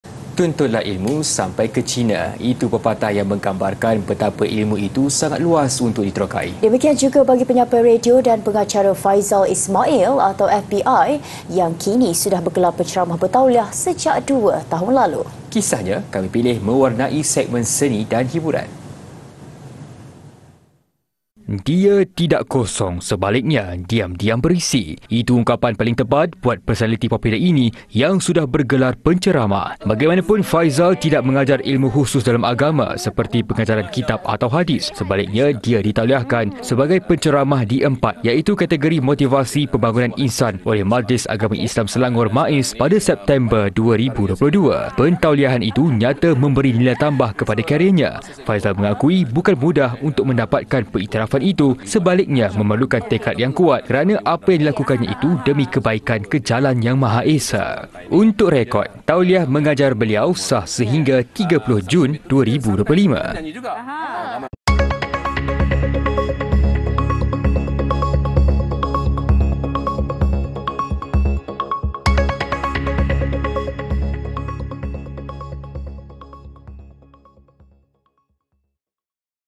FBI Penceramah Bertauliah
Tuntutlah ilmu sampai ke China.Itu pepatah yang menggambarkan betapa ilmu itu sangat luas untuk diterokai.

Demikian juga bagi penyampai radio dan pengacara, Faizal Ismail atau FBI yang kini sudah bergelar penceramah bertauliah sejak dua tahun lalu.